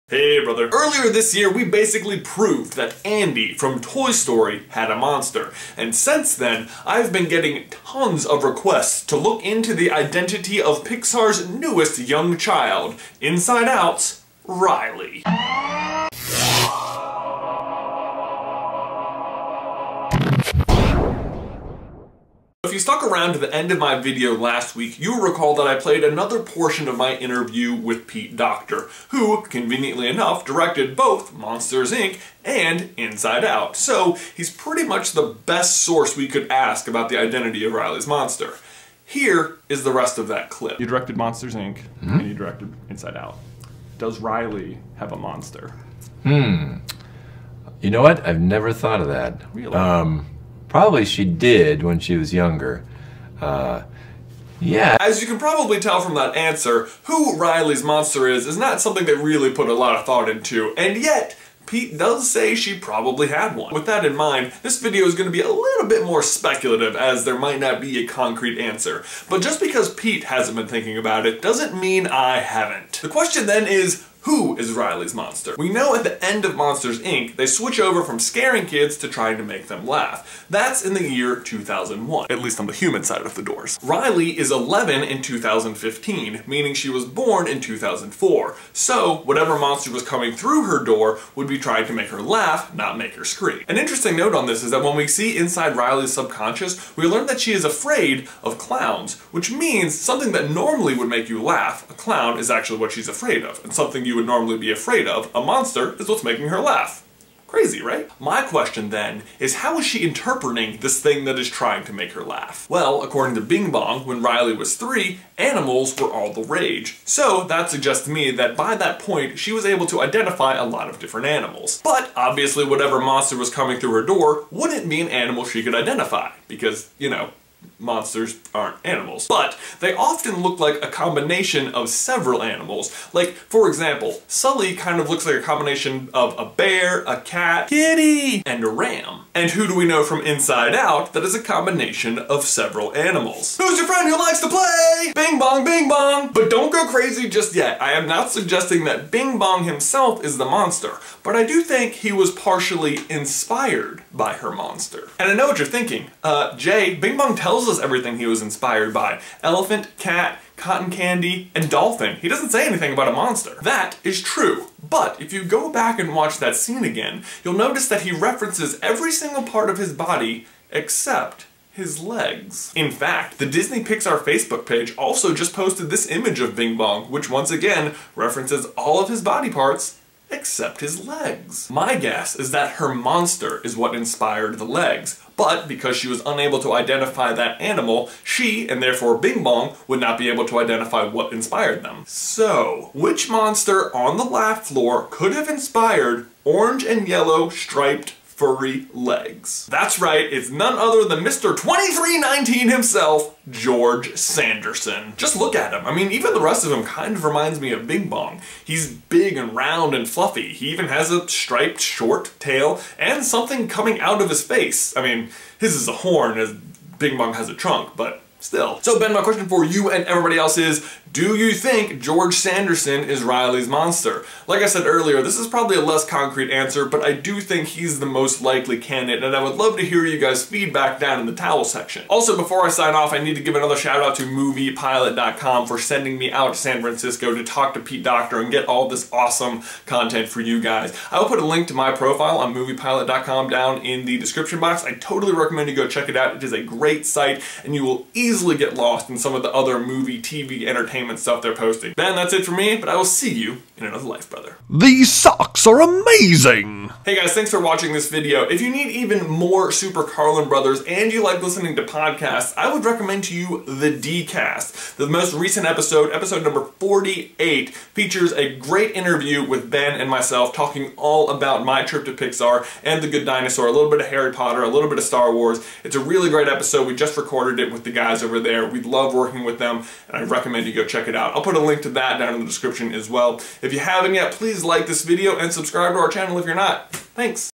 Pixar Theory: Who is Riley's Monster?
What do you think? Is he Riley's Monster??

Every time you give us a thumbs up a Narwhal gets its horn! (maybe..) =D